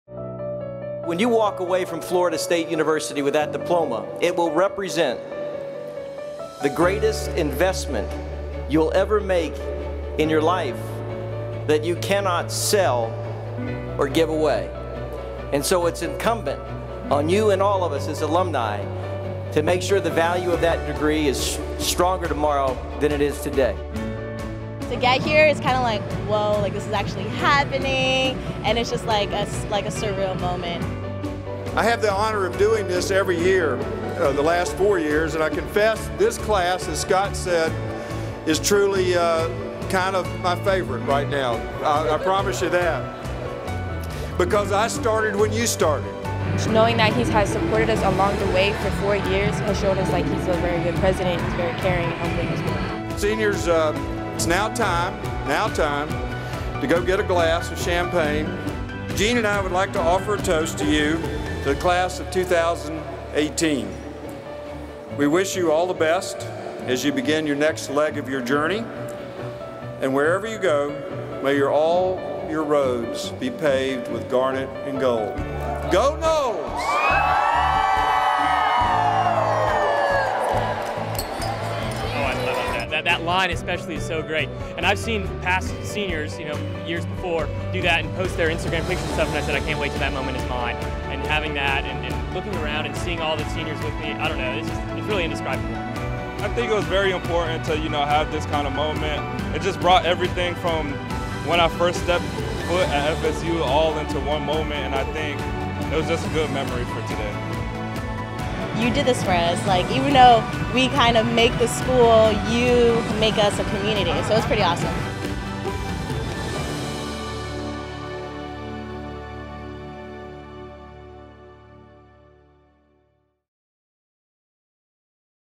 Seniors enjoy a special toast with President Thrasher
Florida State University raises a glass to commemorate the graduating class of 2018 at the annual Senior Toast.